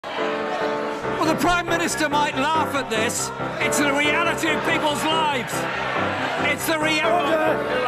"ORDER!" Calm down Jeremy!
"The Prime Minister might laugh at this!" Jeremy Corbyn scolds May on the 5/12/2018 in Parliament during Prime Minister's Question Time, "This is a reality of people's lives!"

He repeats again, shouting at her: "This is the reality of people's lives!"

In response, Theresa shakes her head as the smirk is wiped off her face and the Speaker John Bercow shouts across from the chair, "ORDER!"

A party piece! And somewhat funny, or not, depending upon your standpoint, I suppose!